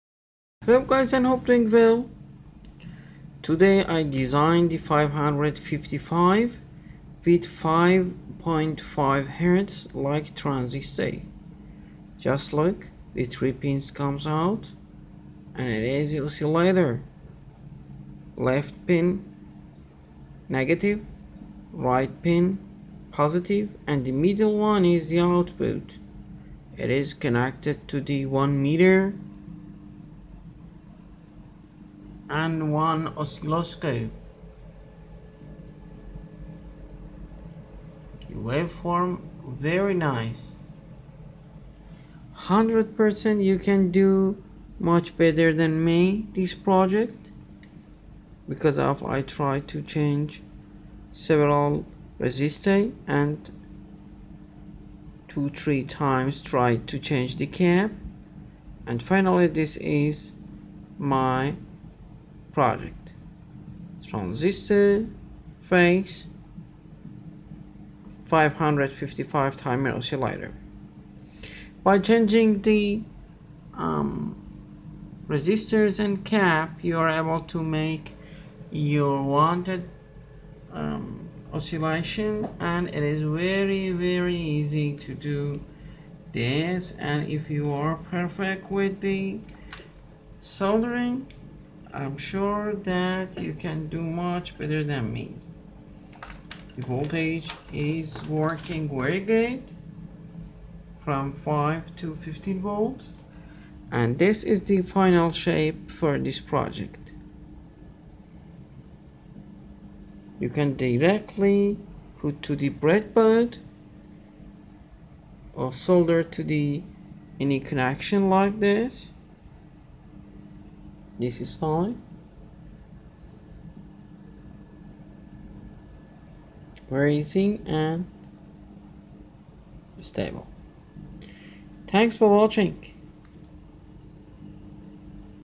Transistor style 555
you can make your needed frequency like this style and use it on pcb or breadboard